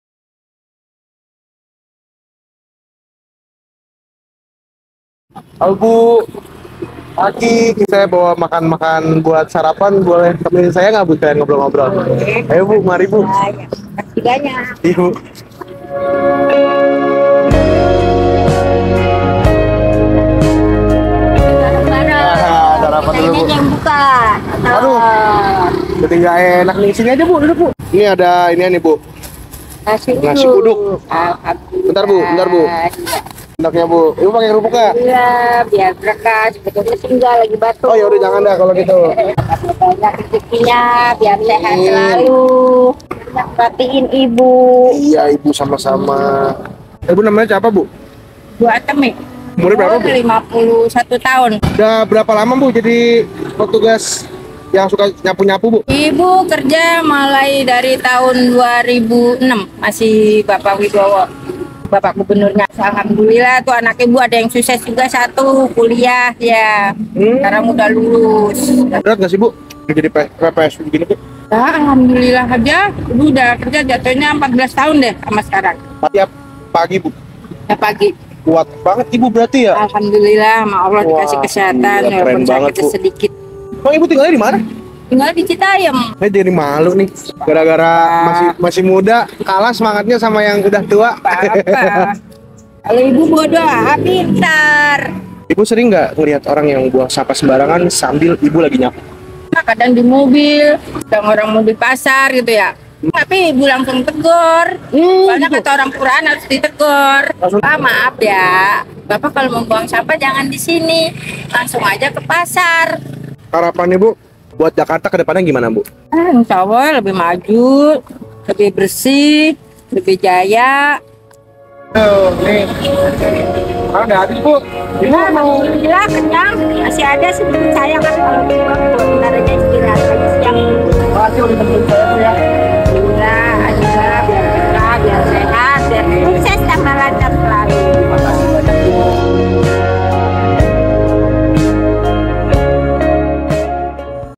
Cerita Atmi di Jakarta...
Kami mengajak makan Atmi, seorang pekerja kebersihan yang masih semangat bekerja di usia senjanya. Ia menceritakan kepada kami perjuangan dan angan-angannya yang sederhana.